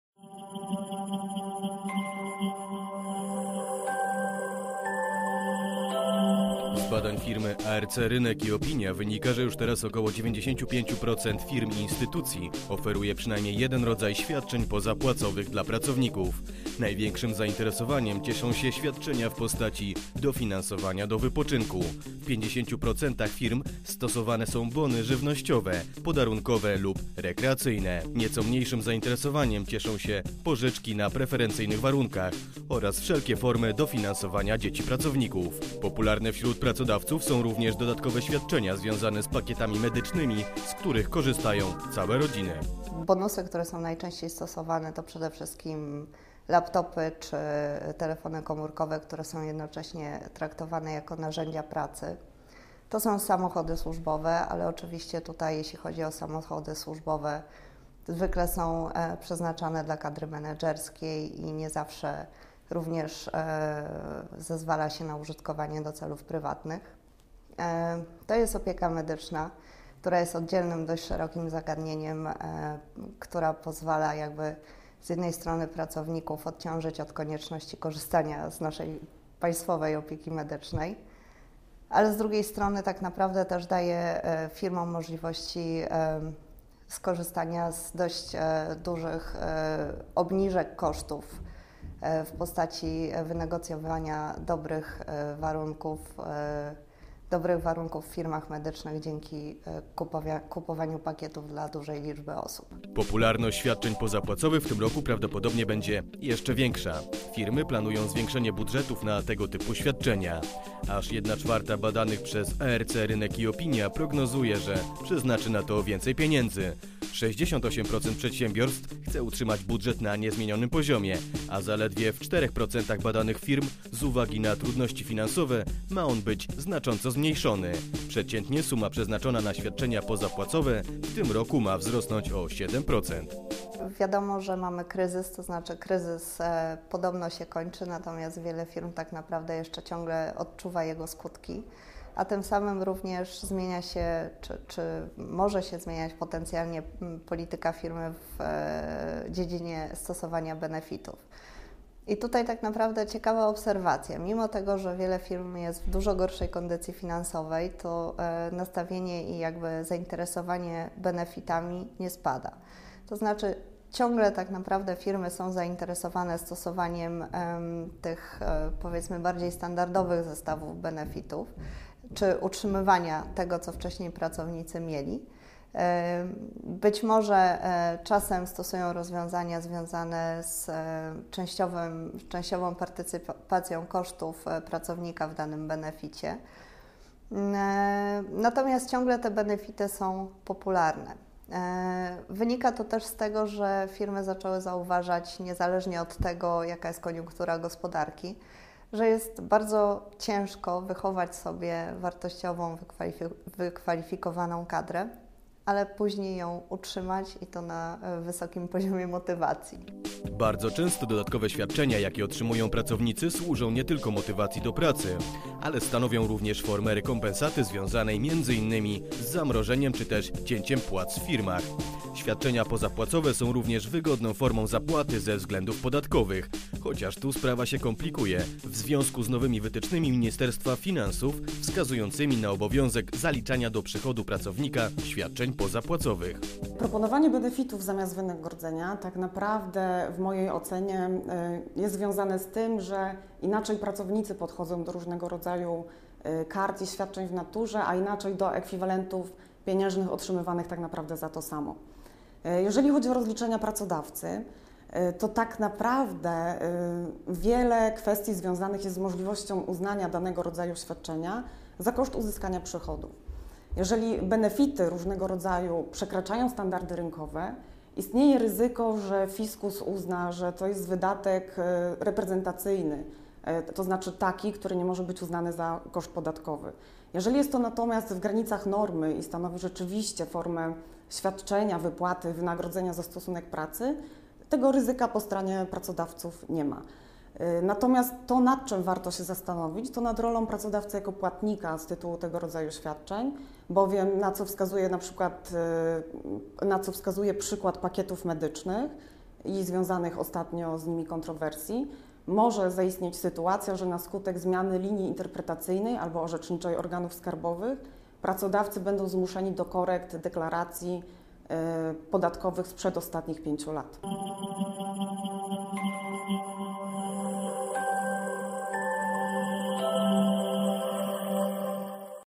Niestandardowe formy wynagradzania pracowników.
Około 95 proc. firm oferuje przynajmniej jeden rodzaj świadczeń pozapłacowych dla pracowników. Największym zainteresowaniem cieszy się dofinansowanie do wypoczynku.